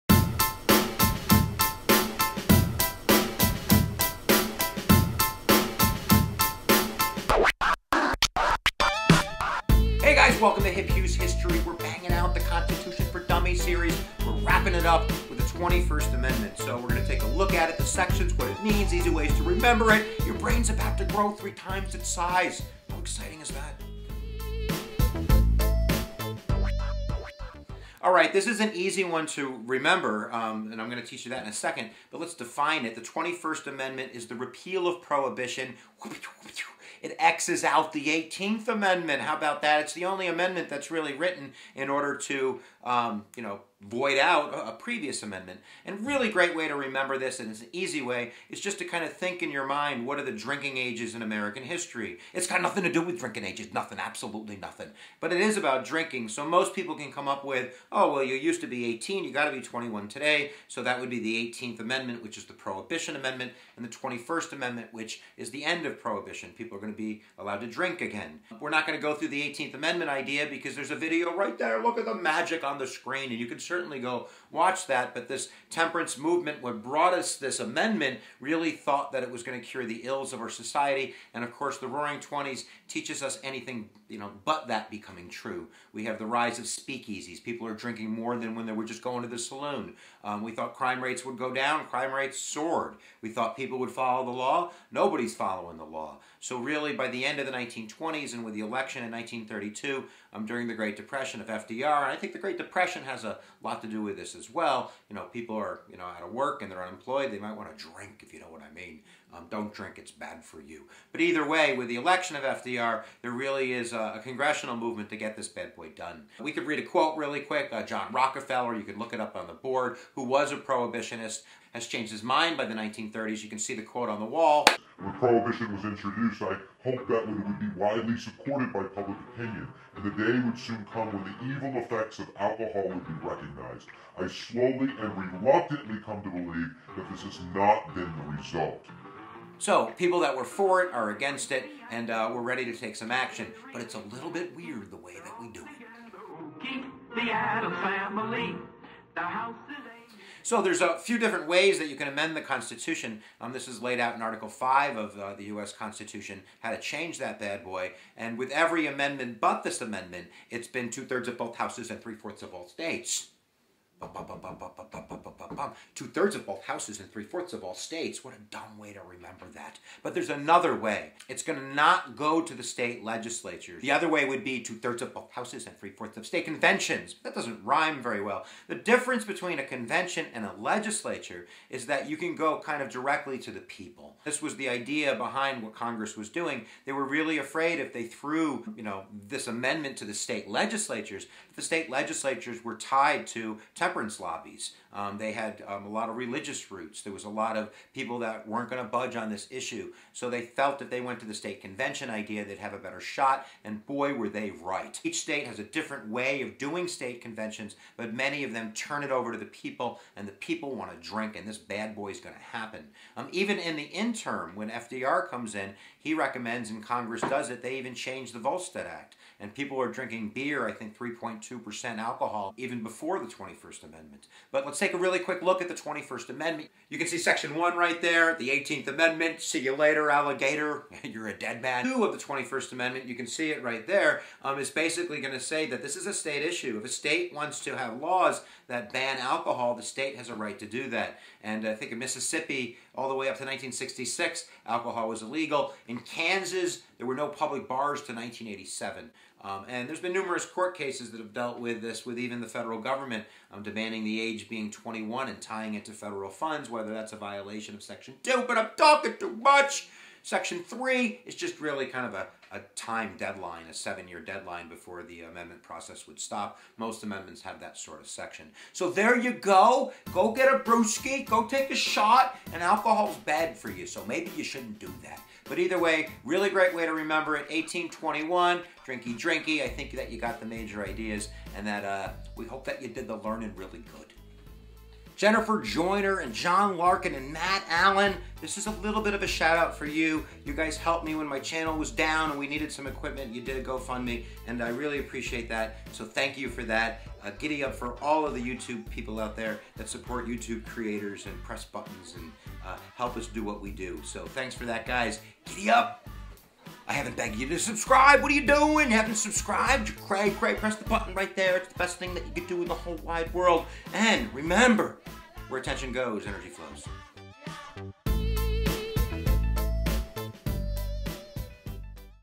The 21st Amendment Explained: American Government Review
Grab a fake a beer and chug down some Constitutional learning. Great for students, life long learners and the cray cray on the internets.